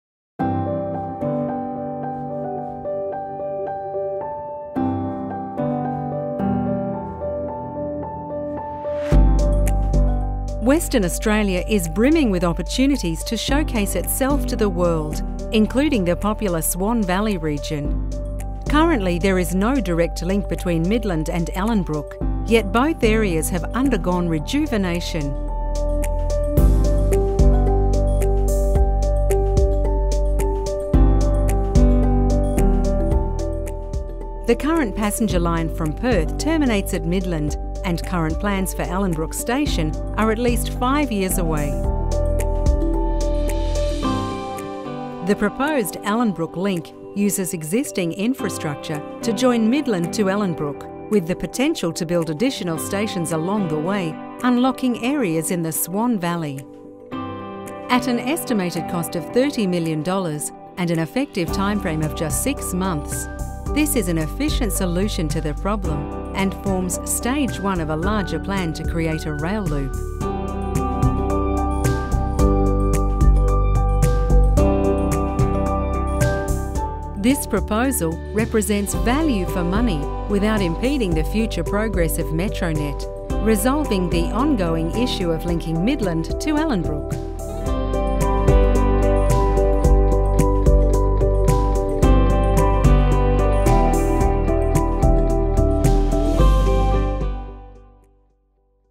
Connect Ellenbrook to Midland NOW
A Swan Chamber of Commerce advocacy project.

Western Australia is brimming with opportunities to showcase itself to the world, including the popular Swan Valley Region.

Currently there is no direct link between midland and Ellenbrook, yet both areas have undergone rejuvenation.

The current passenger line for Perth terminates at midland and current plans for Ellenbrook station are at least 5 years away.

The proposed Ellenbrook link uses existing infrastructure to join midland to Ellenbrook with the potential to build additional stations unlocking areas in the swan valley.

At an estimated cost of $30 million and an effective time frame of just 6 months this is an efficient solution to the problem and forms stage one of the larger plan to create a rail loop.

This proposal represents value for money without impeding the future progress of metronet, resolving the ongoing issue of linking Midland to Ellenbrook.

For more information phone or email the chamber.